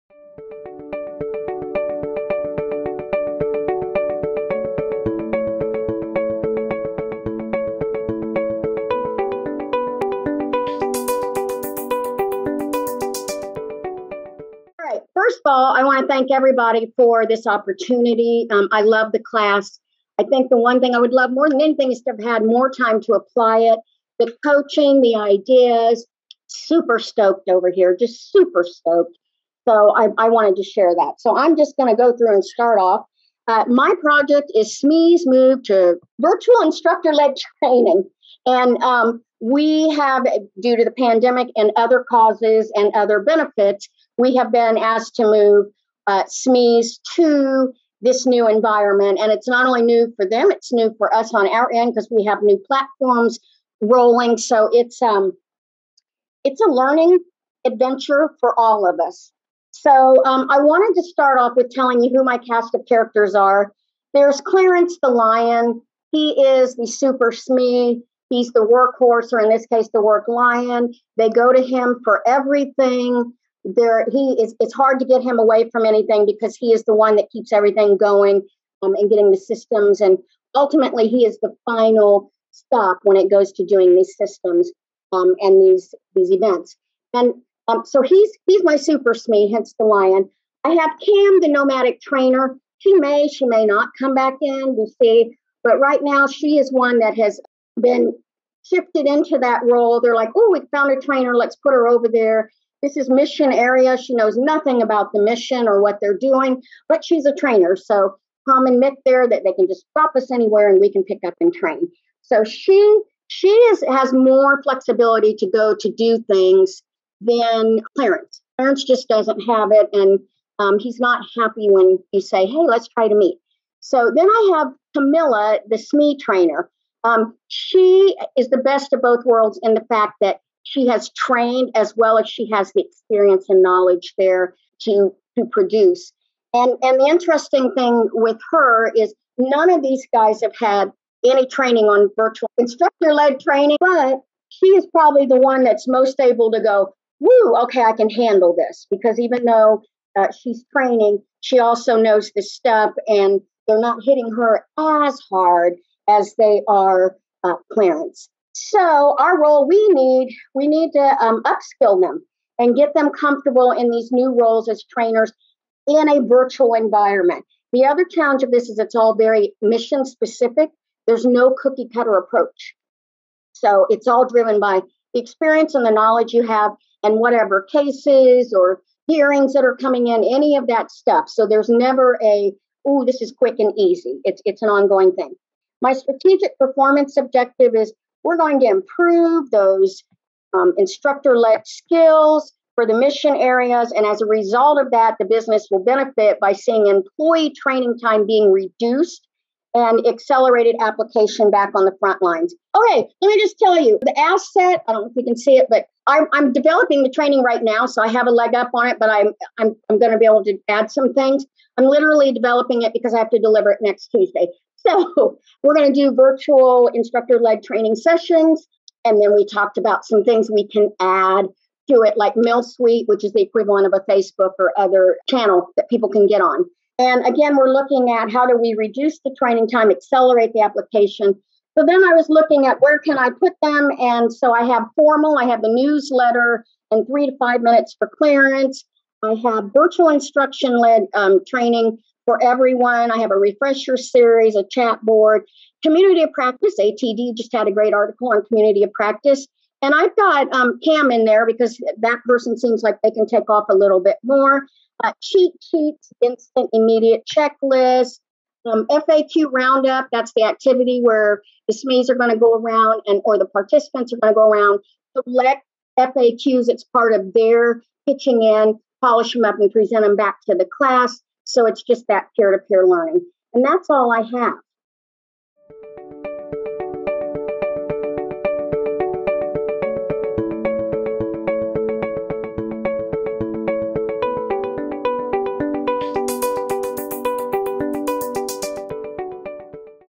Practicum Presentation - Dianne Hanson's Learning Cluster: Moving SMEs to Virtual Training
Dianne Hanson, shares her final product, her Learning Cluster on moving SMEs to Virtual Instructor Led Training, that she developed during the Learning Cluster Design 2-Week Virtual Practicum.